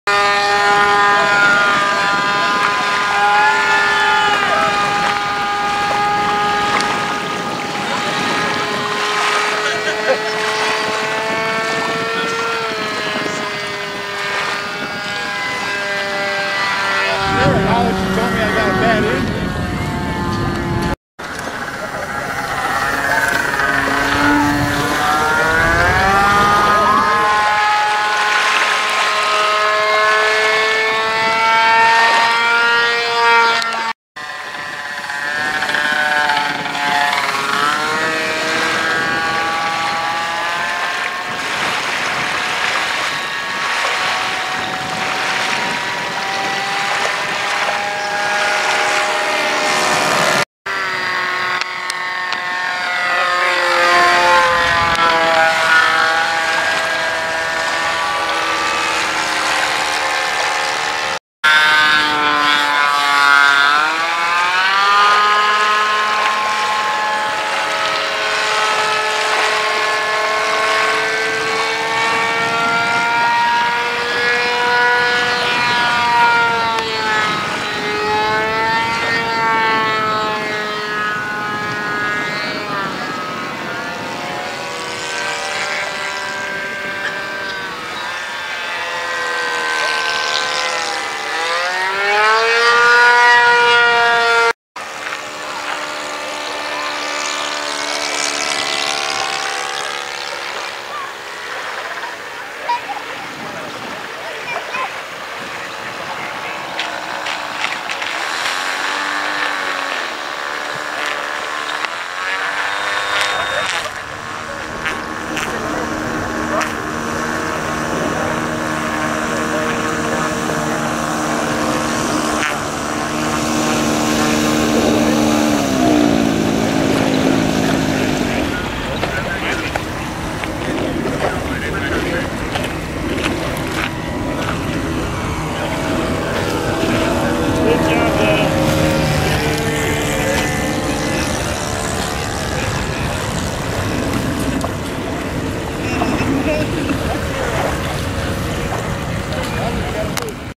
2012-08-09 Denis Sivak's Sig Kadet LT-40
Weather permitting, the Lake Sawyer Hawks fly float planes on the south end of Lake Washington (Seattle, WA) at the Ferdinand Boat Launch Site every Thursday except major holidays from 10:00 am to 1:00 pm.